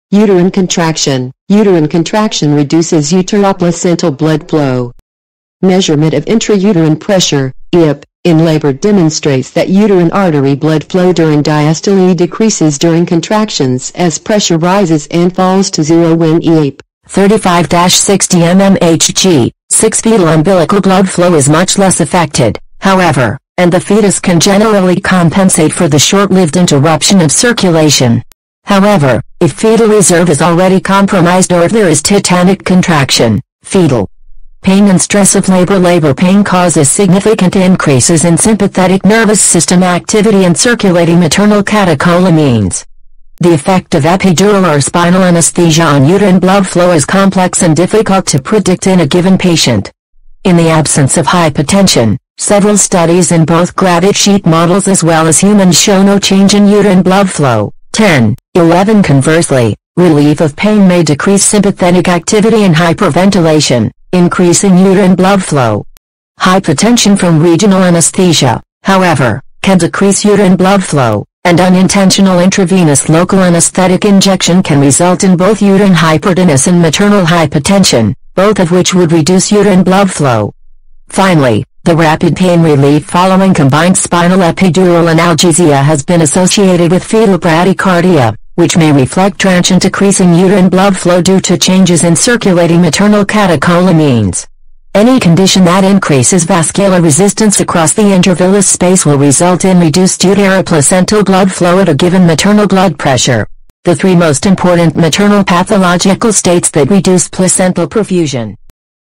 Uterine Contraction
Because the uteroplacental circulation is generally vasodilated, the most important determinant of uteroplacental blood
flow (UBF) is maternal blood pressure in the uterine artery